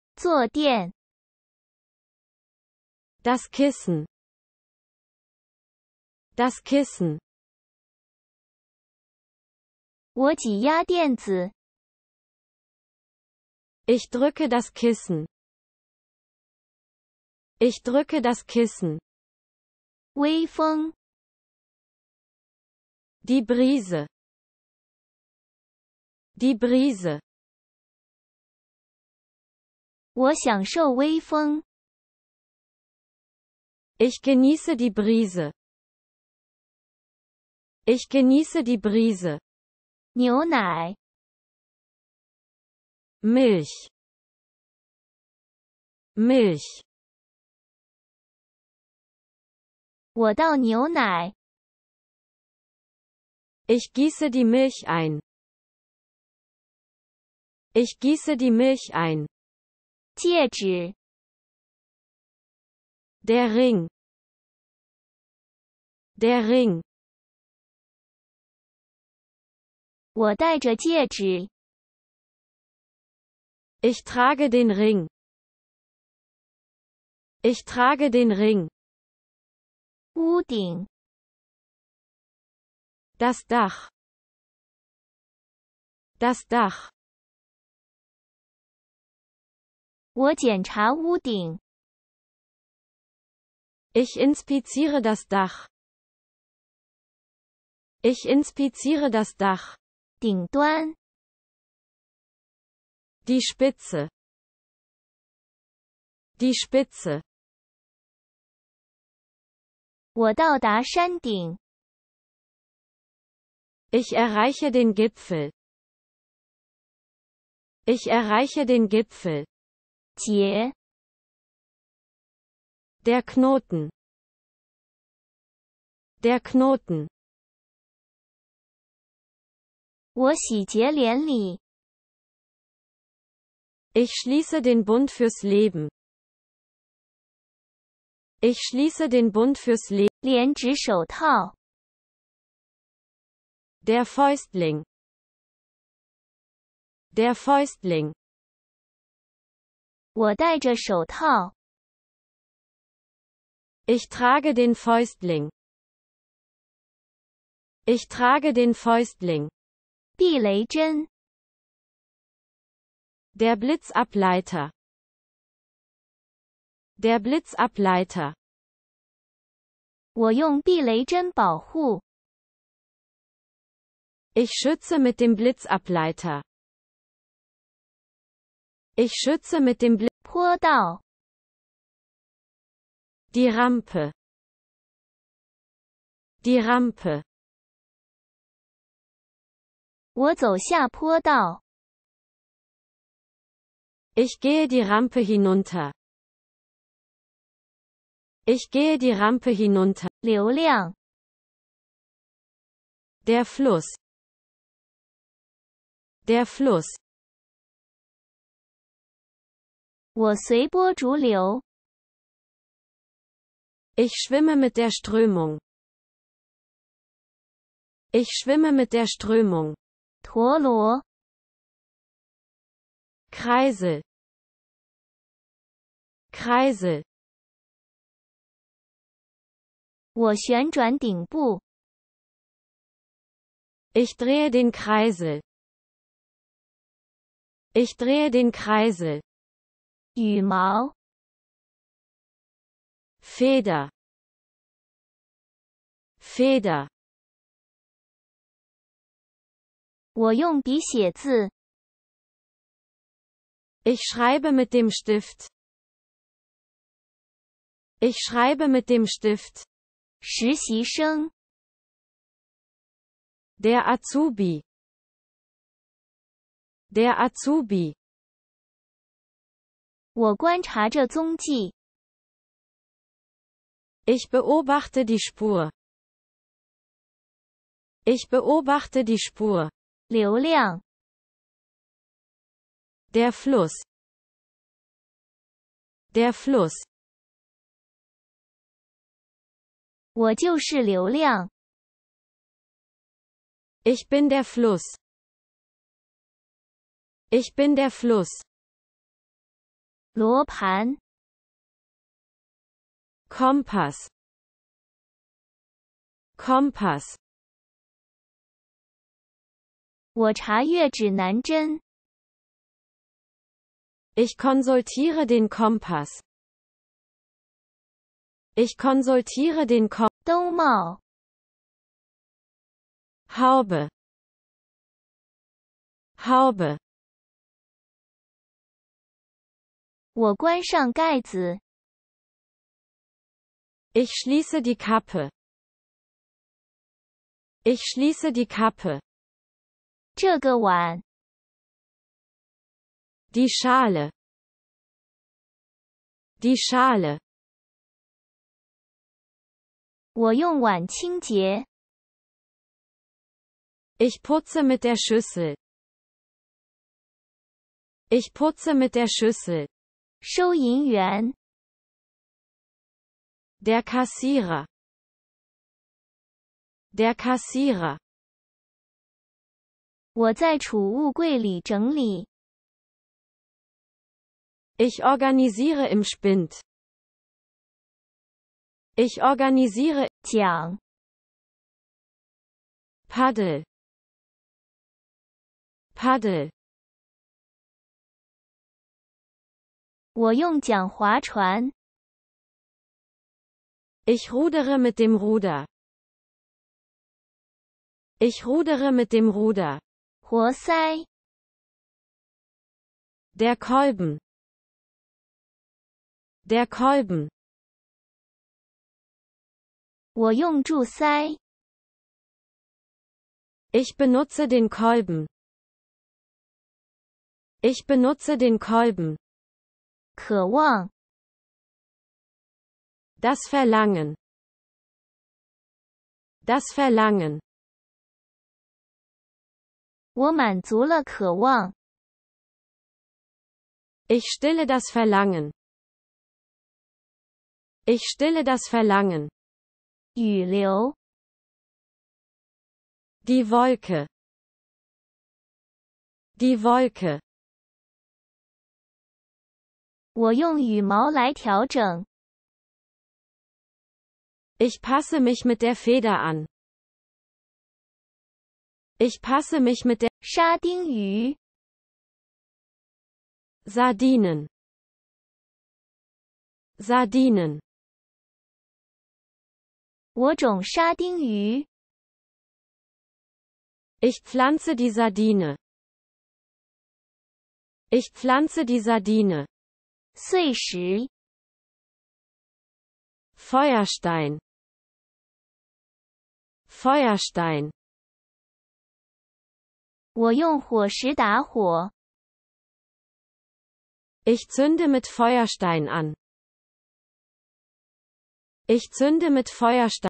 为初学者学习德语。
欢迎来到我致力于为初学者学习德语的 YouTube 频道！在本系列视频中，我将呈现简单的句子，帮助您轻松快速地学习。无论您是想出国旅行、与说英语的人交流还是只是提高德语水平，这些句子都可以让您逐渐发展词汇量和语法。通过我清晰的解释和实际的例子，您将学会在真实的上下文中使用这些句子并轻松记住它们。